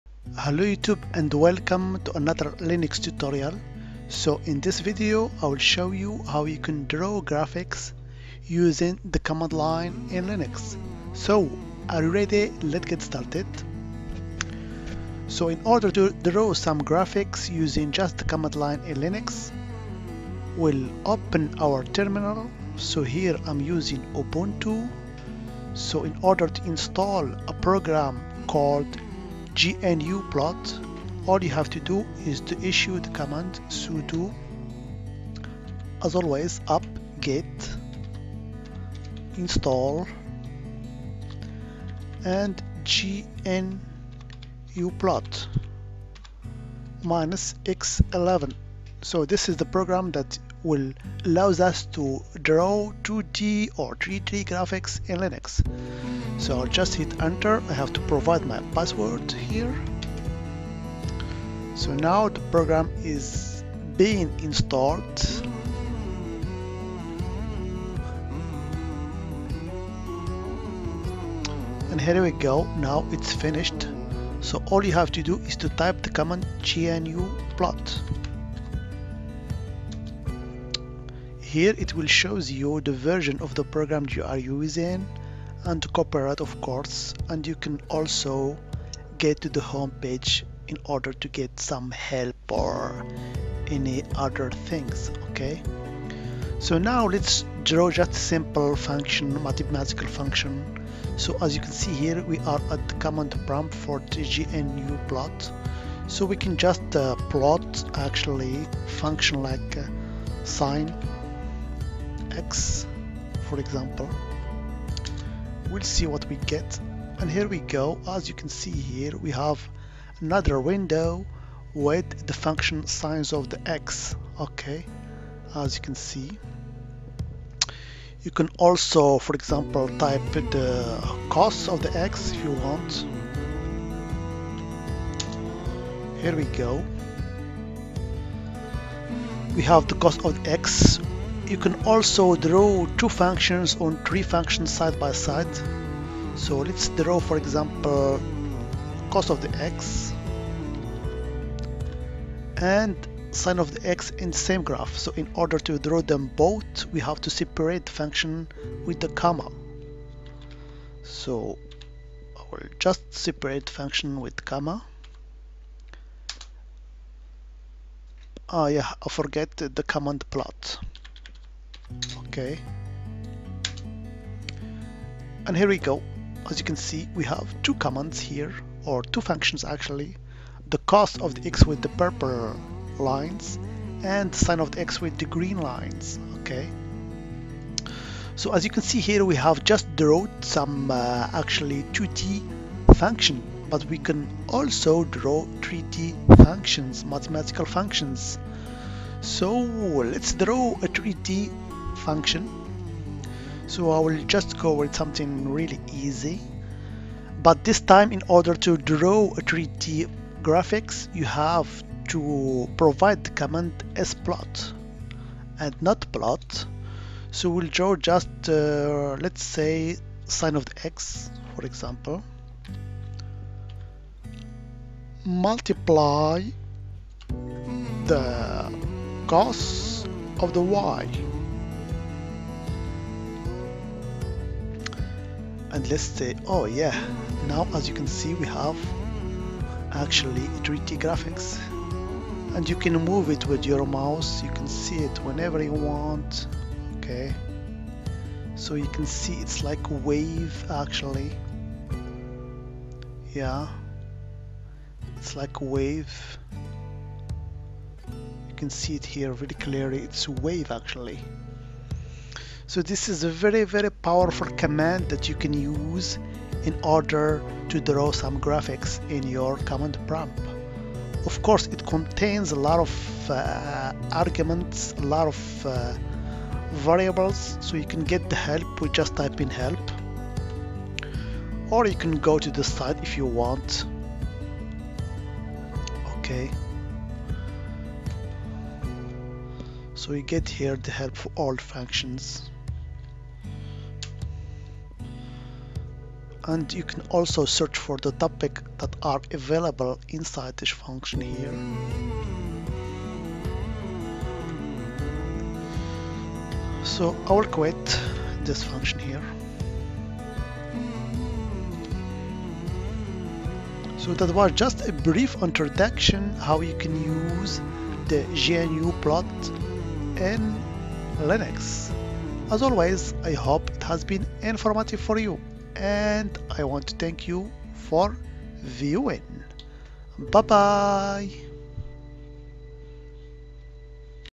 Linux How To Draw 2D & 3D Graphics Using The Command Line
Gnuplot is a portable command-line driven graphing utility for Linux, OS/2, MS Windows, OSX, VMS, and many other platforms.

Gnuplot supports many different types of 2D and 3D plots.
Gnuplot has been supported and under active development since 1986.
gnuplot can produce output directly on screen, or in many formats of graphics files, including Portable Network Graphics (PNG), Encapsulated PostScript (EPS), Scalable Vector Graphics (SVG), JPEG and many others.
The gnuplot core code is programmed in C.